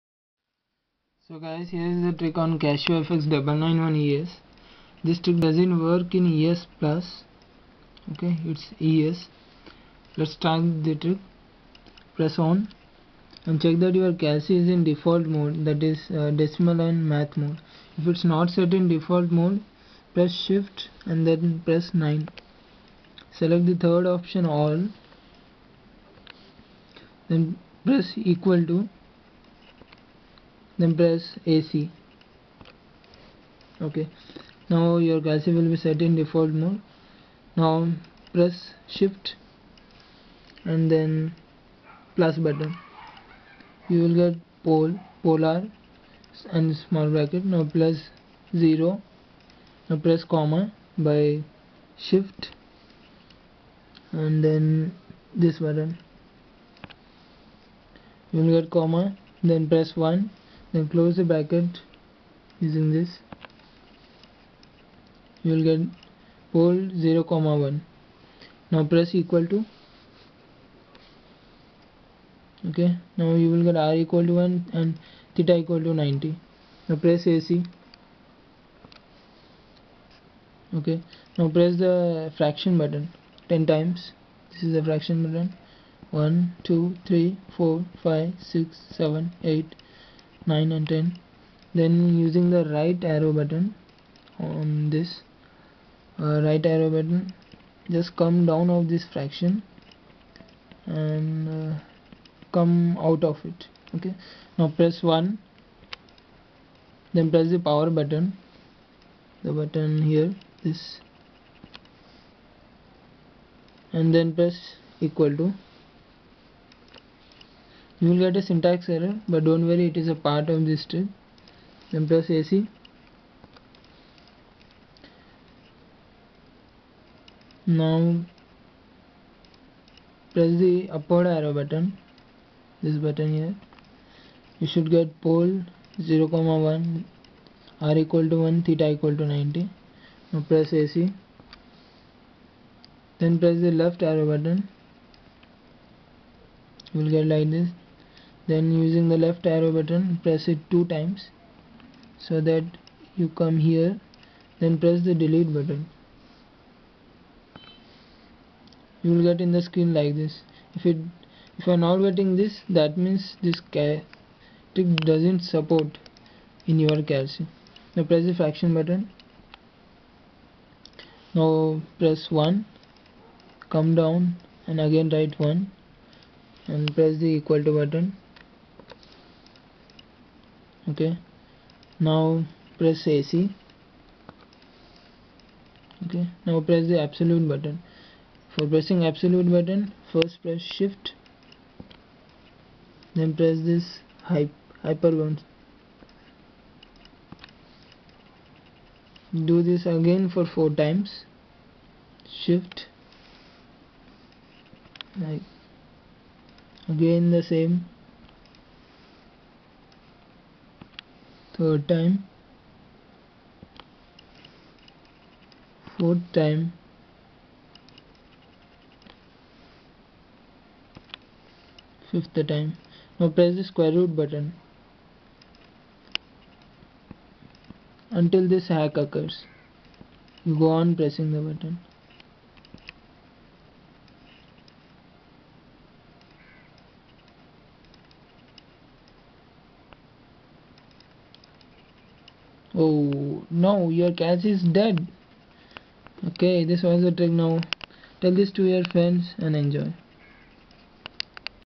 Casio Fx-991ES Trick
Here is a calculator trick on  Casio Fx-991ES Trick to surprise your friends. This trick does not work on Casio Fx-991ES Plus. Follow the instructions as it is.